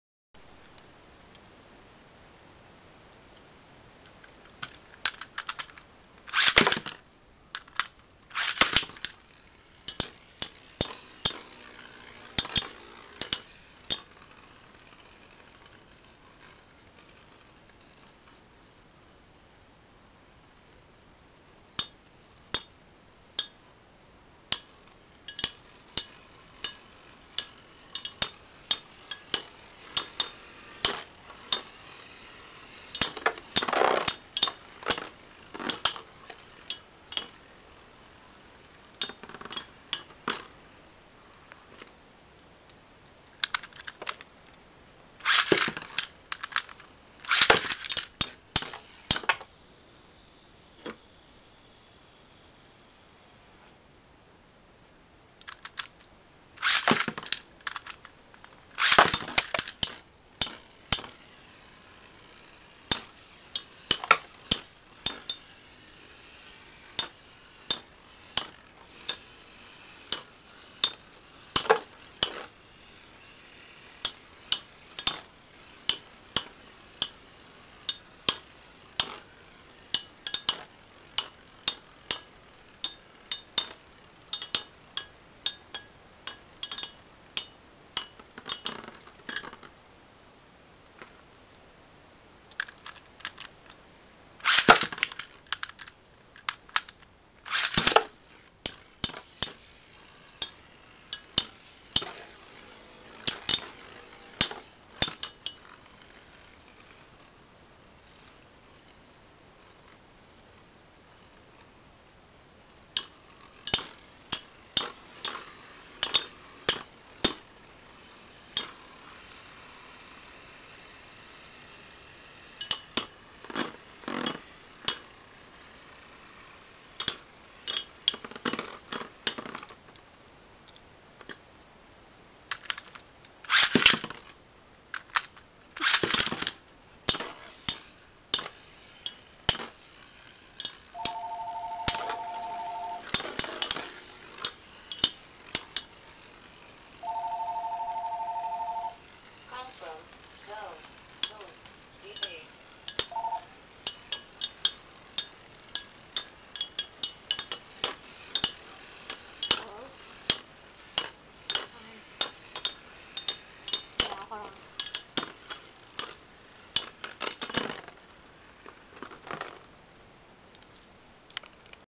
Blitz Unicorno 100RSF V.S Phantom Orion B:D
the unicorno belongs to my friend,epic battle! rate,comment,sub